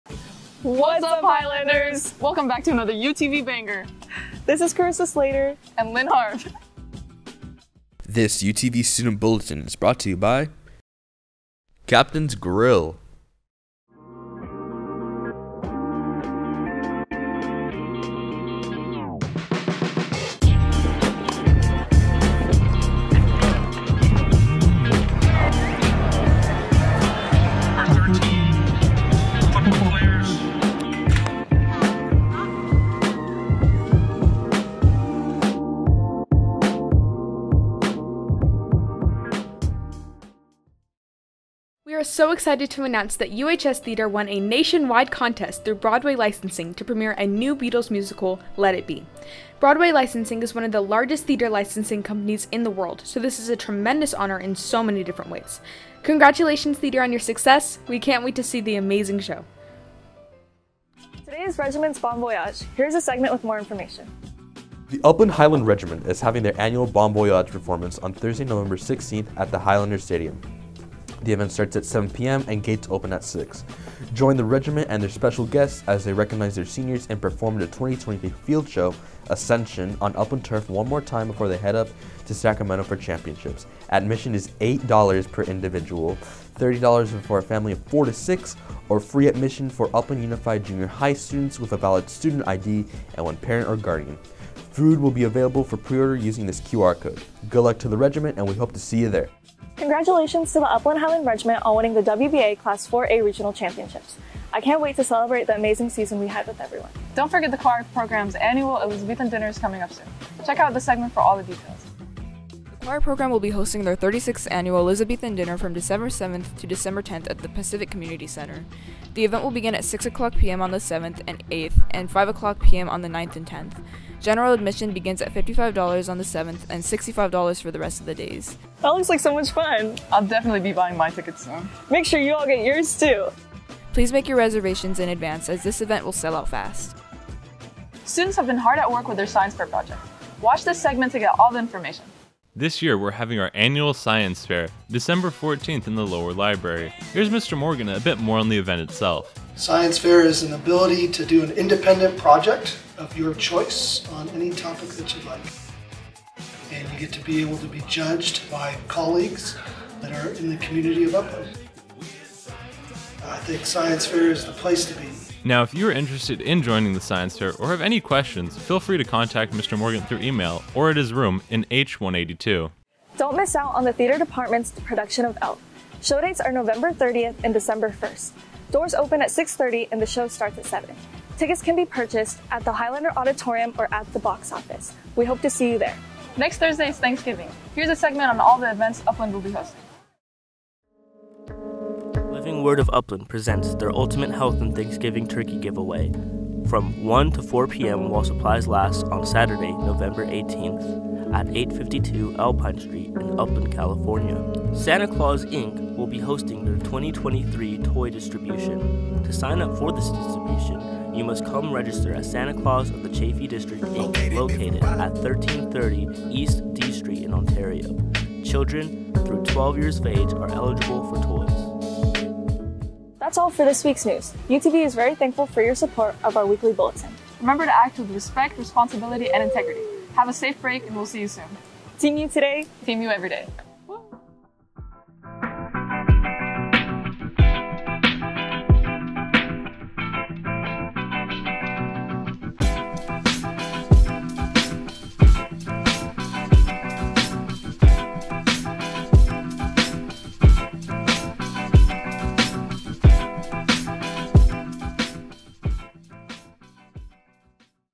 UHS Student Bulletin for November 16, 2023
Enjoy this week's UHS Student Bulletin, produced by Nico Amezcua and edited by Ell Escano. Hosted by Lynn Harb and Carissa Slater.

We hope you have a good Thanksgiving break Highlanders!

📢Special thanks to UTV executive producer Mr. Miura.